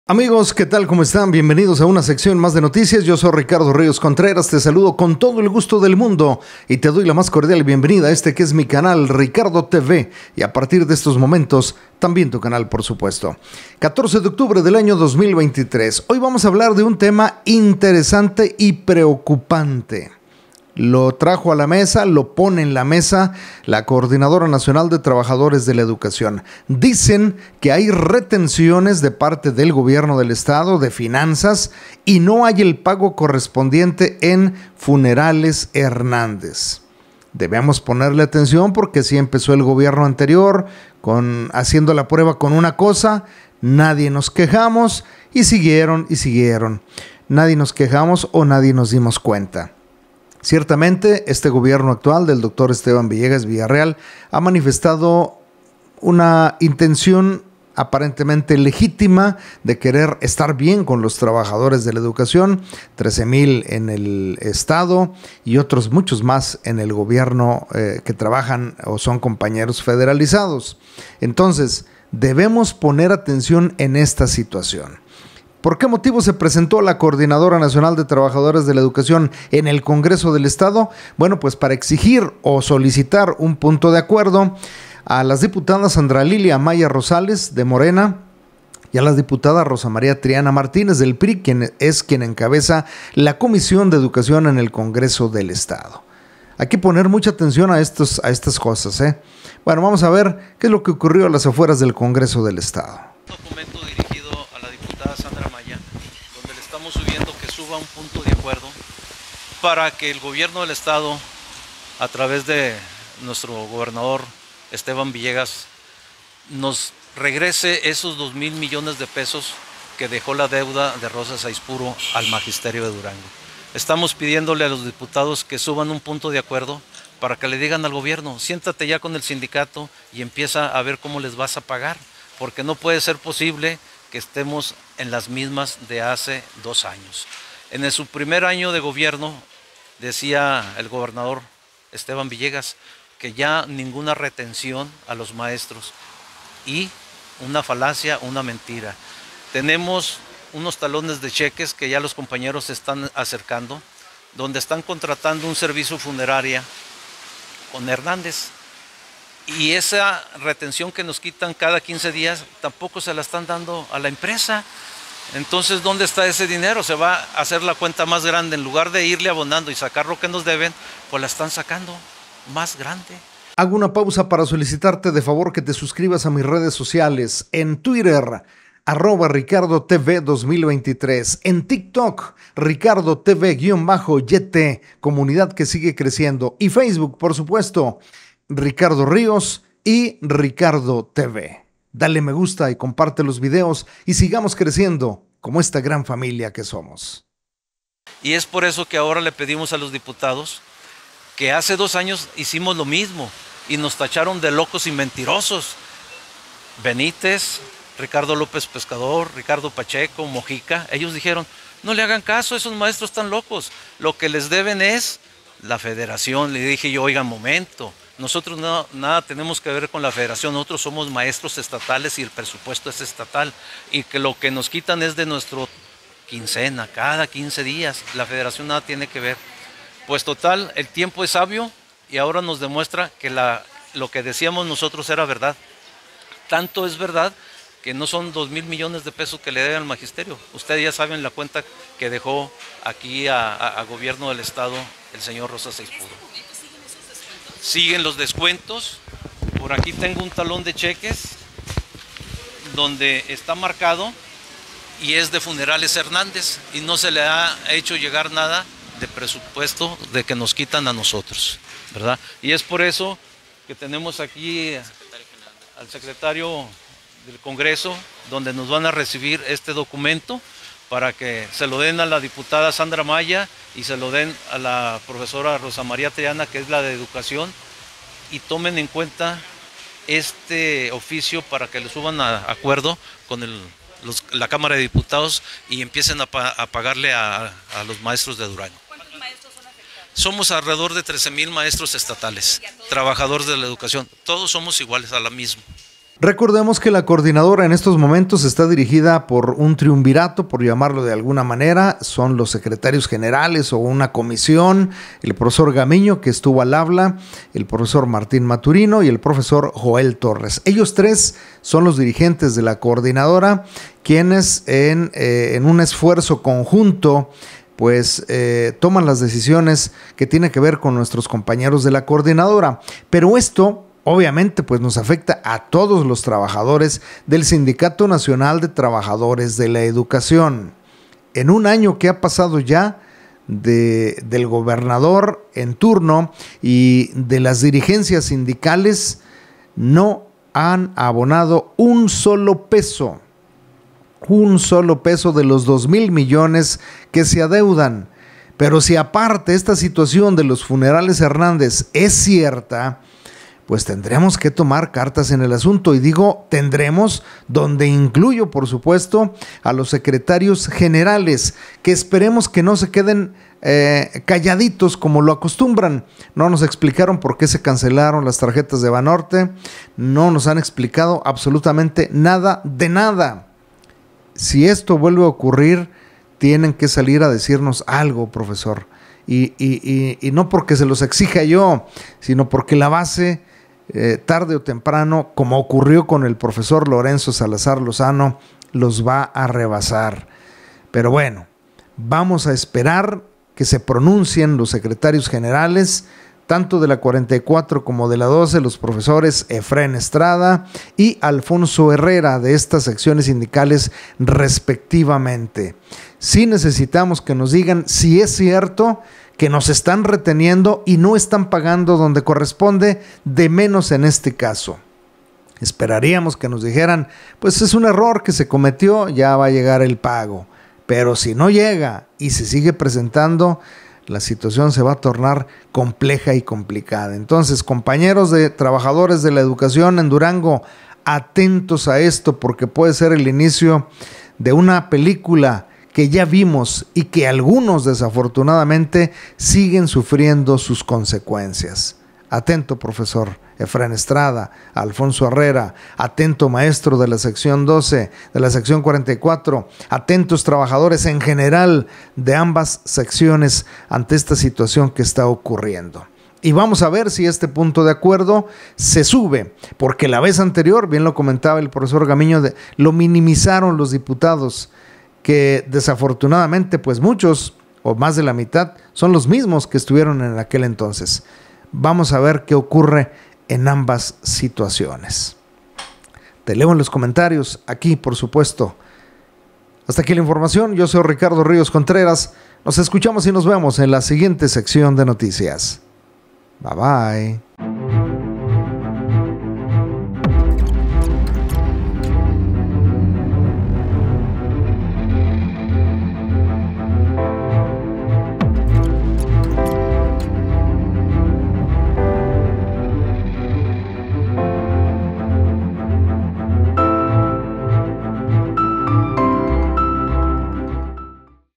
La CNTE exige punto de acuerdo para que se pague los 2 mil mdp y denuncia retenciones no pagadas
Un tema preocupante denuncia la CNTE además de exigir un punto de acuerdo a los diputados del Congreso del estado para que se comience a pagar los 2 mil mdp que se adeudan a los trabajadores de la educación.
FUENTES:
Revisa Zona